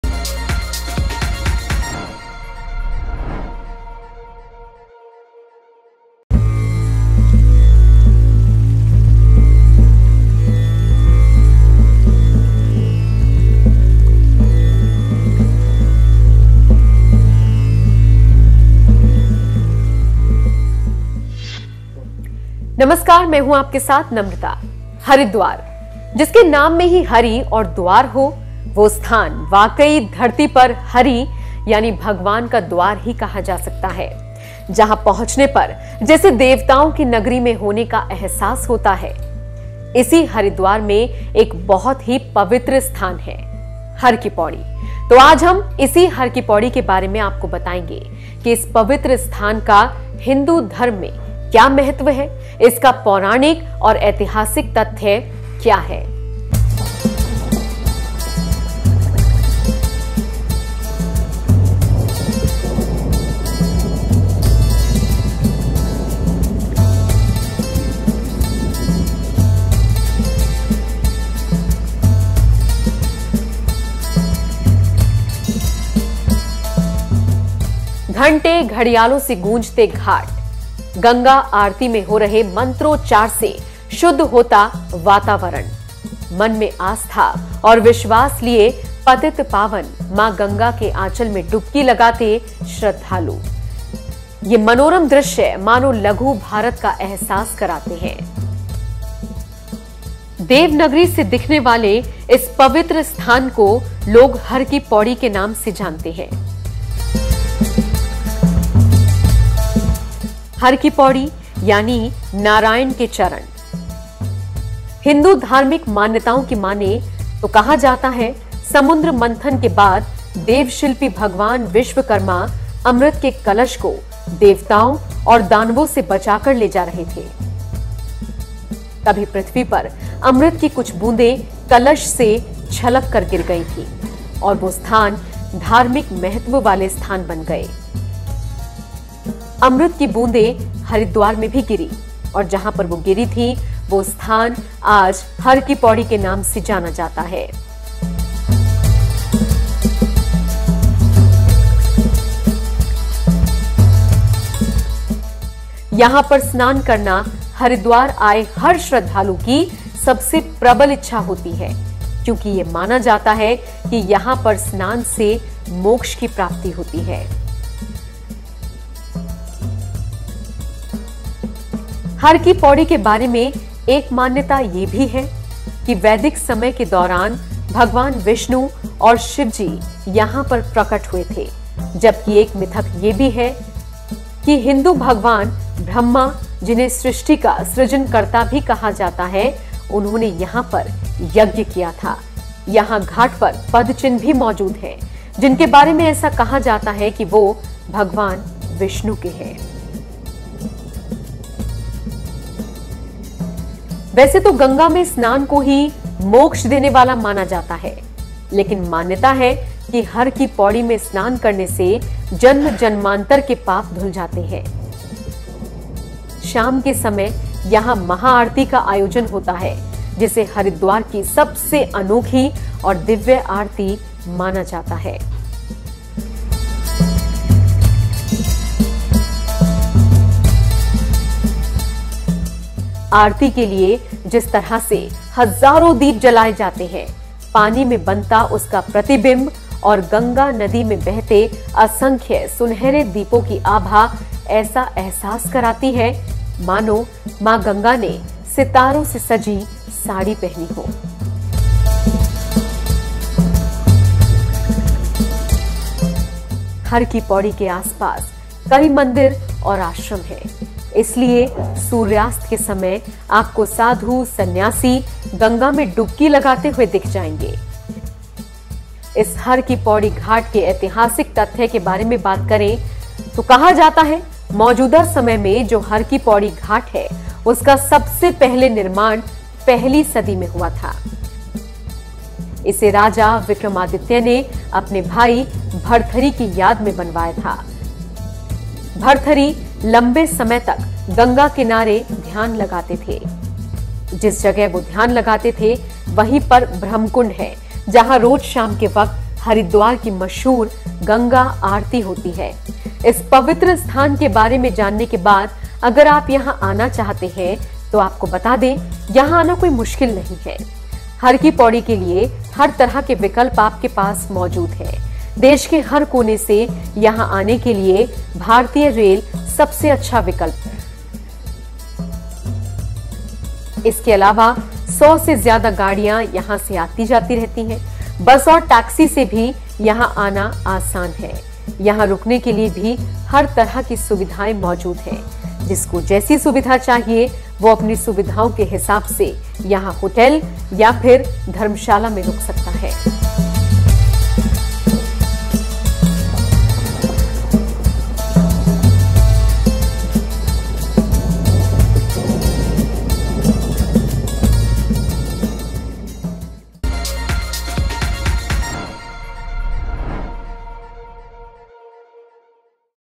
‘हर कि पौड़ी’ को लेकर ये पौराणिक कथा है बेहद दिलचस्प, आप जानते हैं क्या ?
इसी हरिद्वार में एक बहुत ही पवित्र स्थान है... हर की पौड़ी... तो आज हम इसी हर की पौड़ी के बारे में बताएंगे... कि इस पवित्र स्थान का हिंदू धर्म में क्या महत्व है... इसका पौराणिक और ऐतिहासिक तथ्य क्या है !